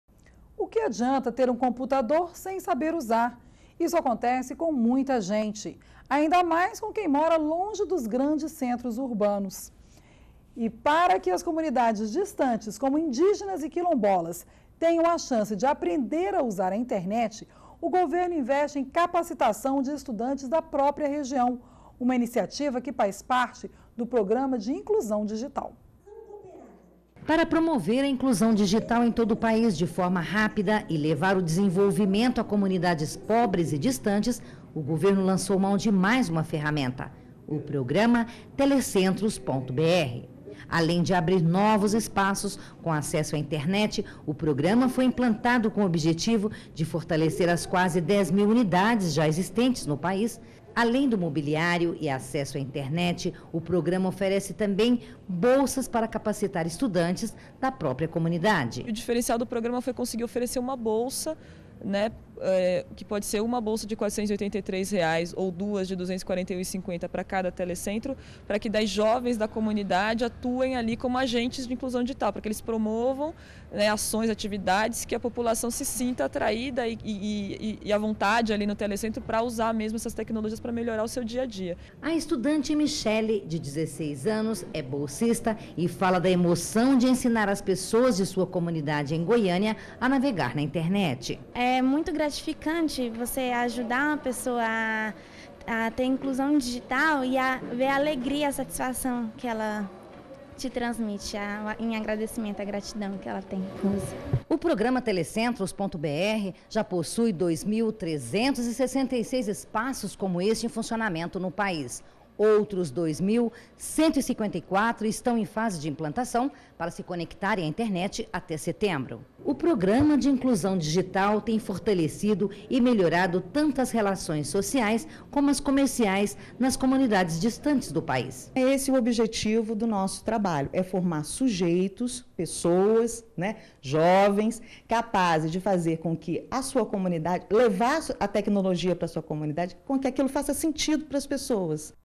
Governo investe em capacitação digital para estudantes de comunidades distantes de grandes centros
NBR NOTÍCIAS - 14.04.12: O governo federal está investindo em capacitação de estudantes que moram longe dos grandes centros, como indígenas e quilombolas, para que tenham a chance de aprender a usar a internet. A capacitação será por meio do Programa de Inclusão Digital.